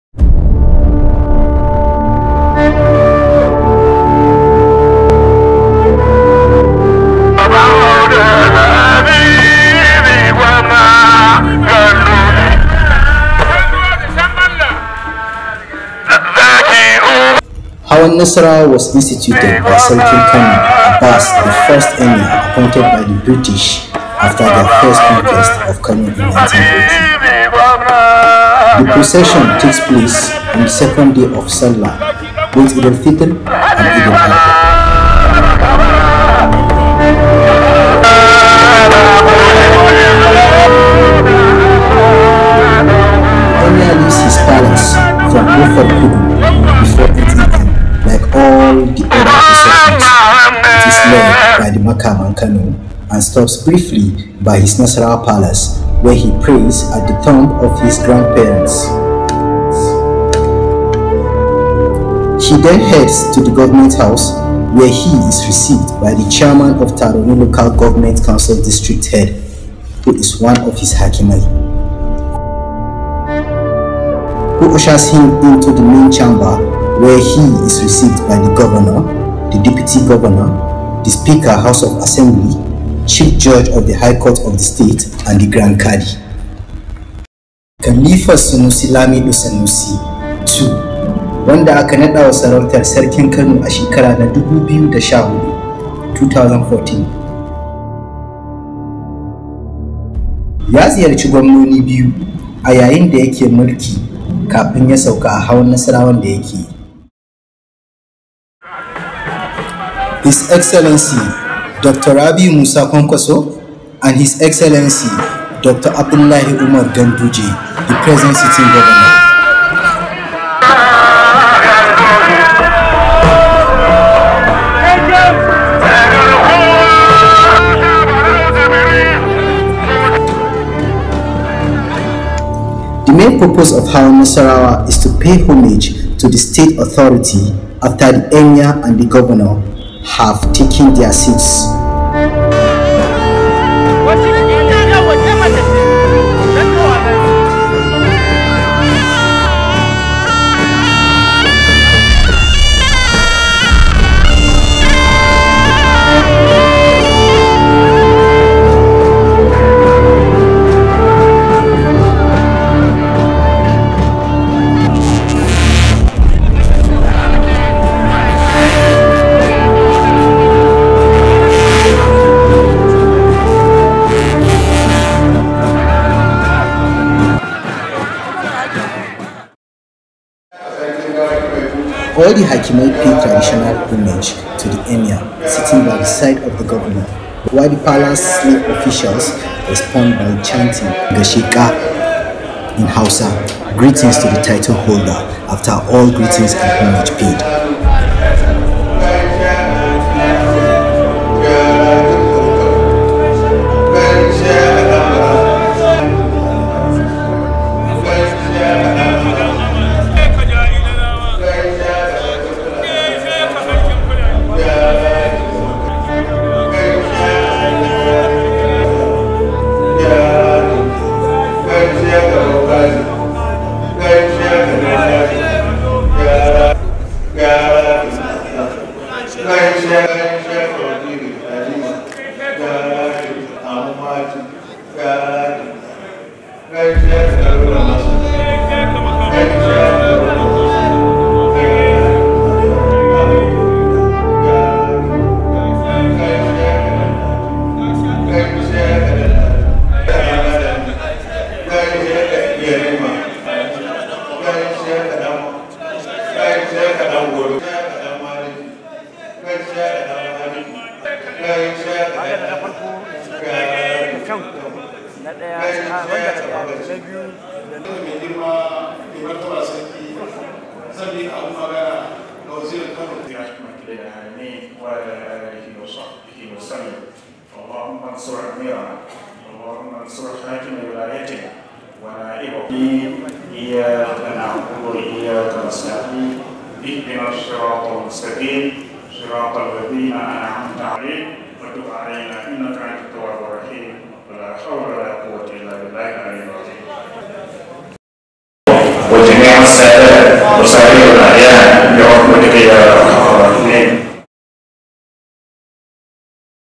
HAWAN NASARAWA PART 1Khalifa SANUSI LAMIDO SANUSI II
The last Durbar(hawan nasarawa) KHALIFA SANUSI LAMIDO SANUSI II
KANO NIGERIA